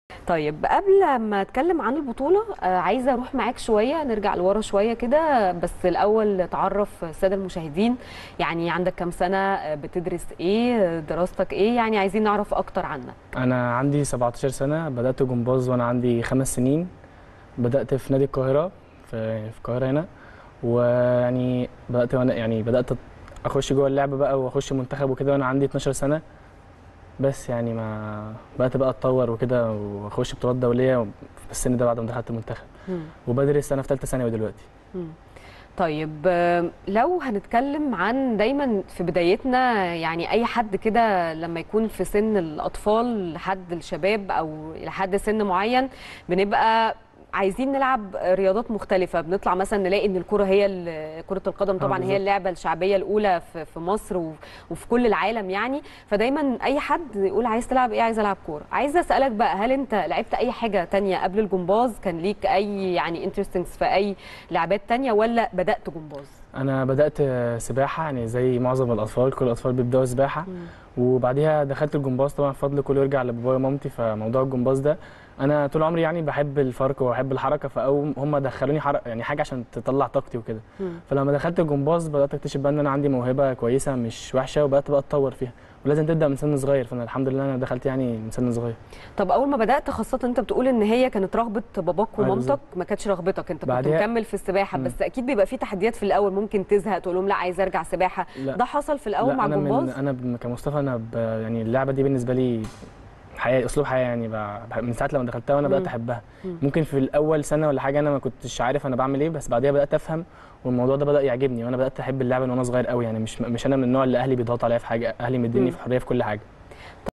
مصطفى وليد لاعب المنتخب للجمباز الفني : اكتشفت موهبتي في سن صغير وأصبح الجمباز بالنسبة لي اسلوب حياة
مصطفى وليد - لاعب منتخب مصر للجمباز الفني : اكتشفت موهبتي في سن صغير وأصبح الجمباز بالنسبة لي اسلوب حياة